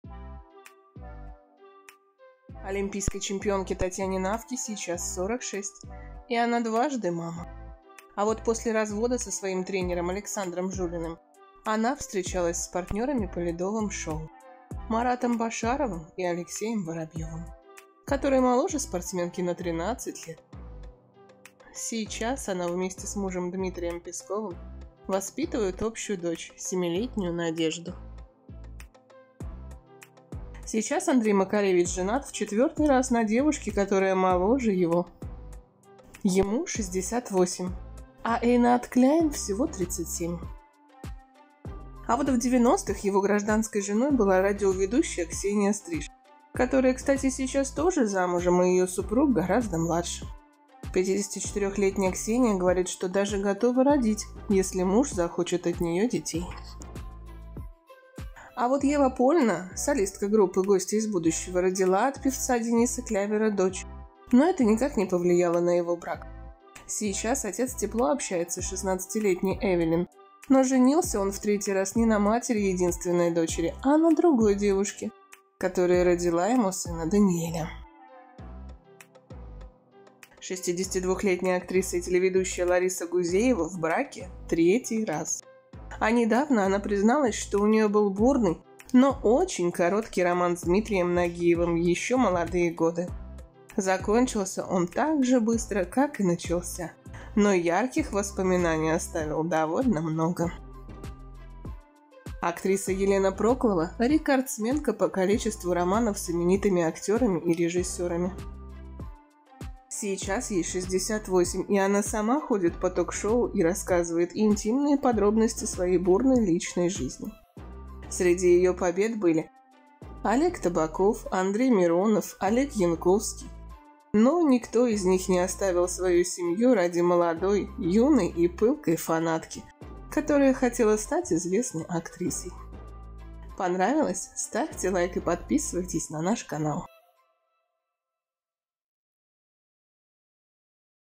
5 пар знаменитостей, которые на самом деле встречались
Олимпийской чемпионке Татьяне Навке сейчас 46 лет и она дважды мама. А вот после развода со своим тренером Александром Жулиным, она встречалась с партнерами по ледовым шоу - Маратом Башаровым и Алексеем Воробьевым, который моложе спортсменки на 13 лет. Сейчас она вместе с мужем Дмитрием Песковым воспитывает общую дочь 7-летнюю Надежду.

Сейчас Андрей Макаревич женат в четвертый раз на девушке, которая моложе его. Ему 68 лет, а Эйнат Кляйн всего 37. А вот в 90-х его гражданской женой была радиоведущая Ксения Стриж, которая, кстати, сейчас тоже замужем и ее супруг гораздо младше. 54-летняя Ксения говорит, что даже готова родить, если муж захочет от нее детей.

А вот Ева Польна - солистка группы "Гости из будущего" родила от певица Дениса Клявера дочь, но это никак не повлияло на его брак. Сейчас отец тепло общается с 16-летней Эвелин, но женился он в третий раз не на матери единственной дочери, а на другой девушке, которая родила ему сына Даниэля.

62-летняя актриса и телеведущая Лариса Гузеева в браке третий раз. А недавно она призналась, что у нее был бурный, но очень короткий роман с Дмитрием Нагиевым в еще молодые годы. Закончился он так же быстро, как и начался, но ярких воспоминаний оставил довольно много.

Актриса Елена Проклова - рекордсменка по количеству романов с именитыми актерами и режиссерами. Сейчас ей 68 лет и она сама ходит по ток-шоу и рассказывает интимные подробности своей бурной личной жизни. Среди ее "побед" были: Олег Табаков, Андрей Миронов и Олег Янковский. Но никто из них не оставил свою семью ради молодой, юной и пылкой фанатки, которая хотела стать известной актрисой.